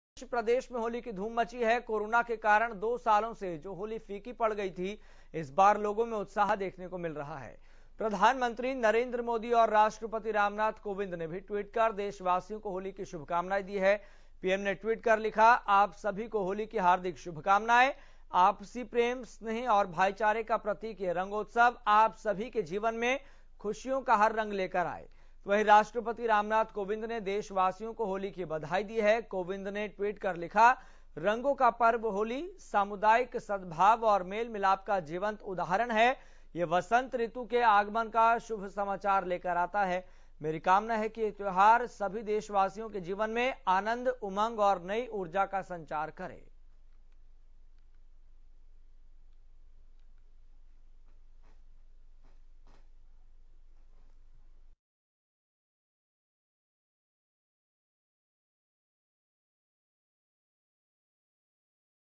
पीएम मोदी ने दी होली की शुभकामनाएं, ट्वीट कर दिया ये खास संदेश
आज देश-प्रदेश में होली की धूम मची है. कोरोना के कारण दो सालों से जो होली फीकी पड़ गई थी.. इस बार लोगों में उत्साह देखने के लिए मिल रहा है. वहीं प्रधानमंत्री नरेंद्र मोदी और राष्ट्रपति रामनाथ कोविंद ने भी ट्वीट कर देशवासियों को होली की शुभकामनाएं दी है.